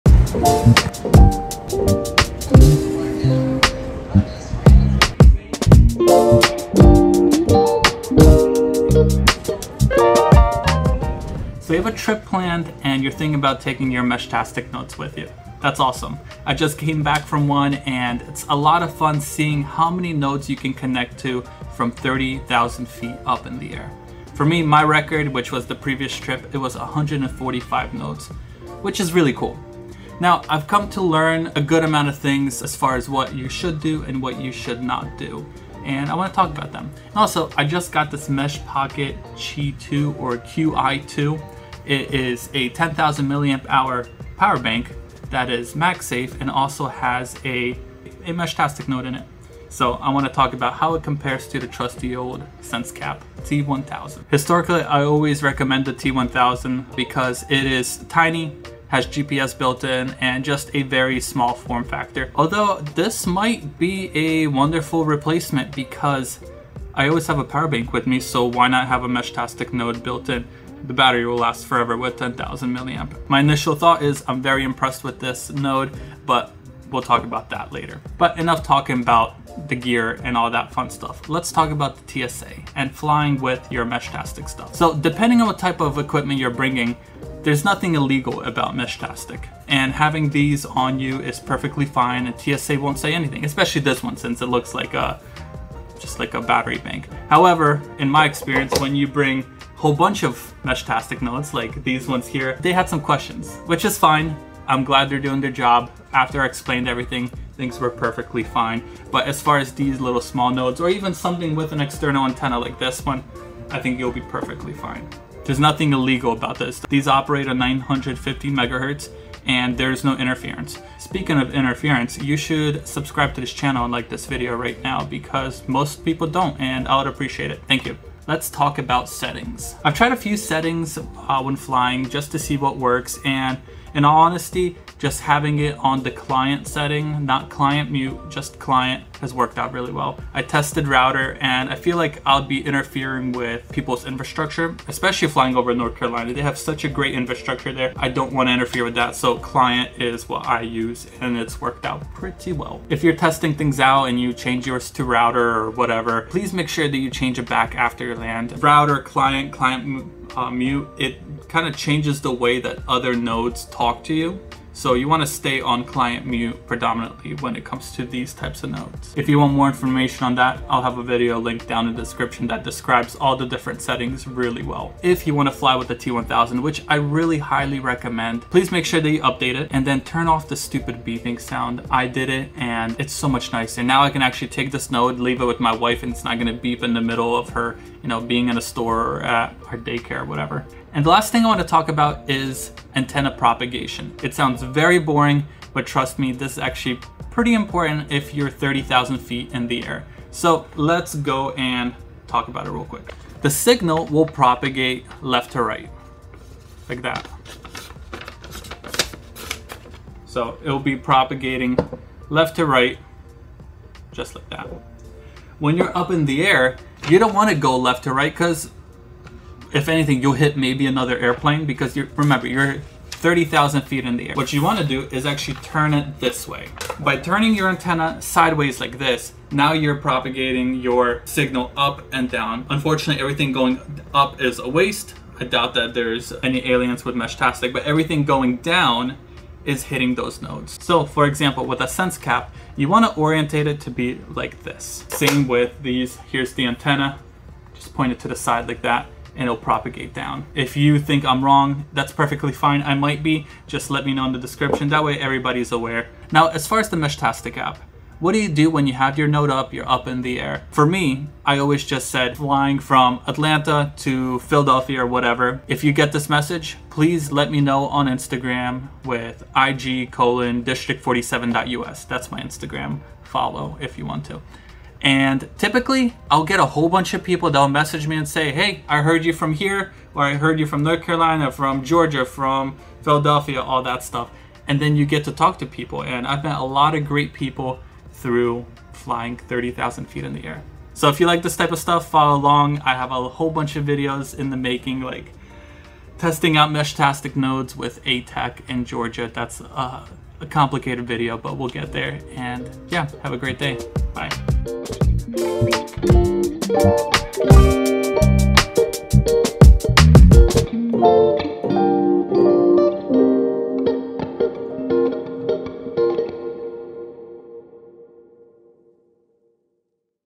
Tips & Tricks to Flying With a Meshtastic Node
With a few thousand miles behind me, I've had a chance to take my Meshtastic nodes along with me. Here's a few tips and tricks that I've learned with flying with a Meshtastic Node.

 Your results may vary, but I'd love to know how it works out for you!!!

🧰 Nodes I talked About:
Meshpocket Qi2
Sensecap T-1000e

⏱️ Video Chapters:
00:00 – Intro
01:10 – Parts List
03:21 – Build Guide
05:51 – Flashing the Firmware
09:20 – Battery Configuration
10:40 – Solar Panel Overview
11:12 – Outro